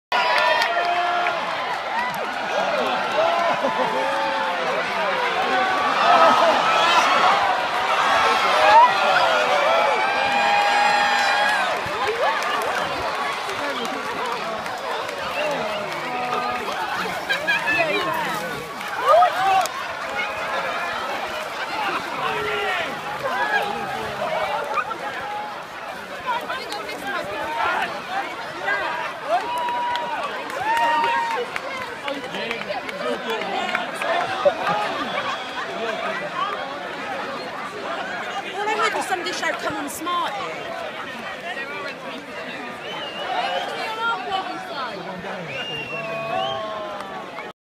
Michael Attwooll smart fog
The dragon making his way down the hill at the 2014 cheese rolling!